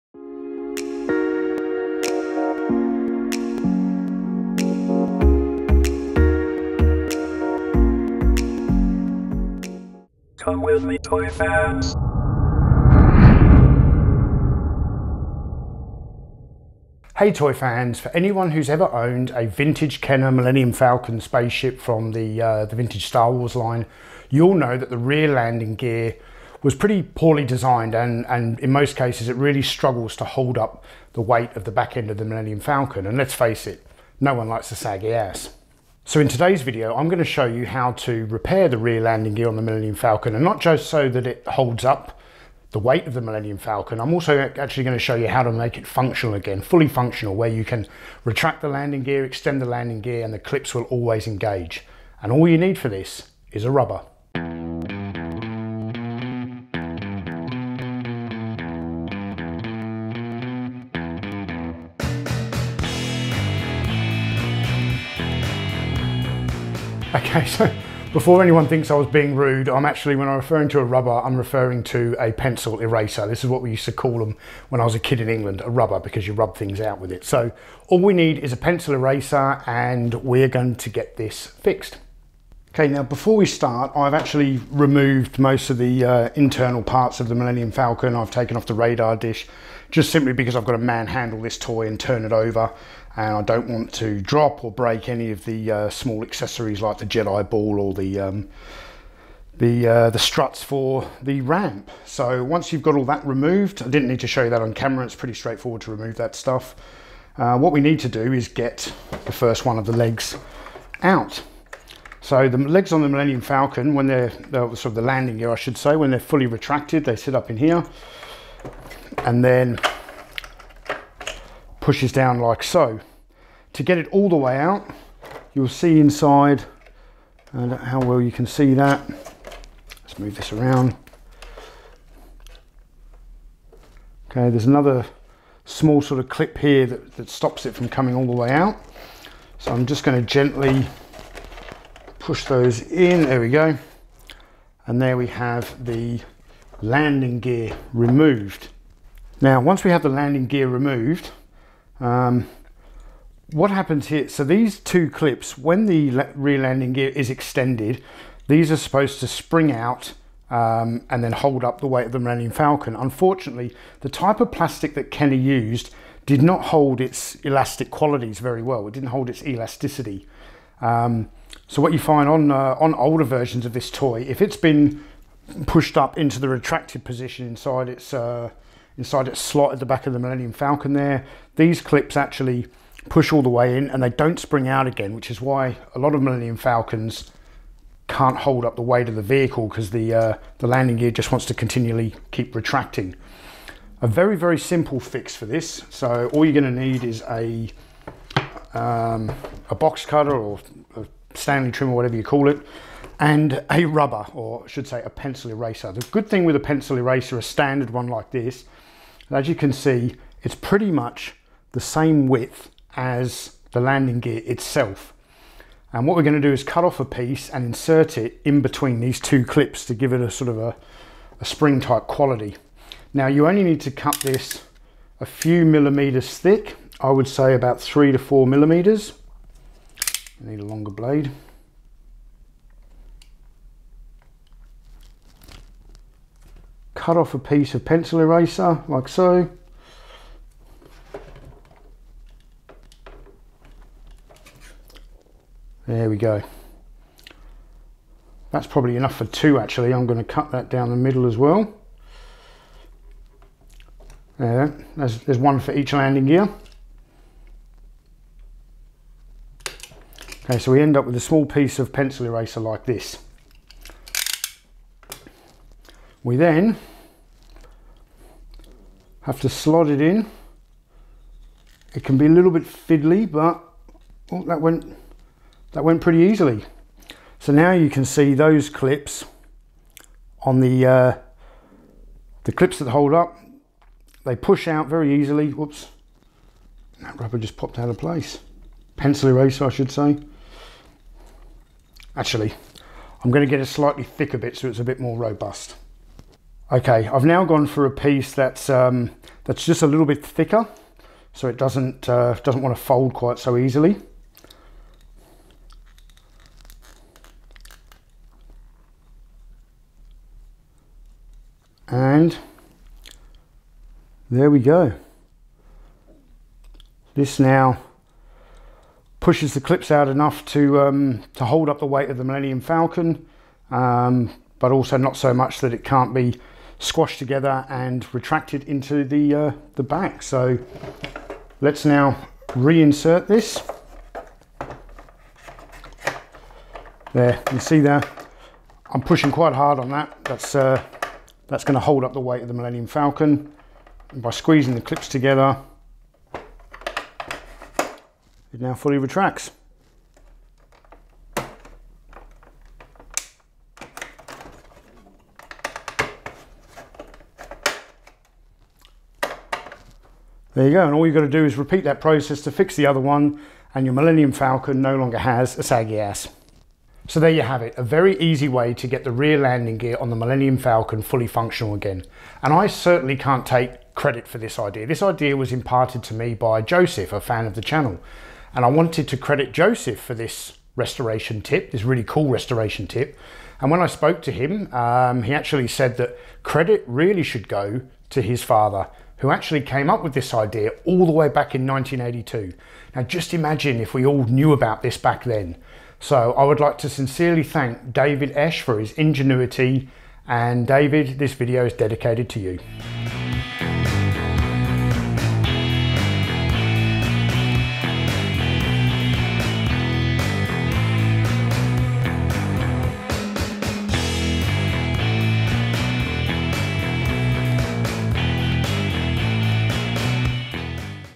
Kenner Millennium Falcon - How to repair the landing gear!
Our Channel includes vintage toy reviews, restoration tips and featurettes about the hobby of vintage toy collecting.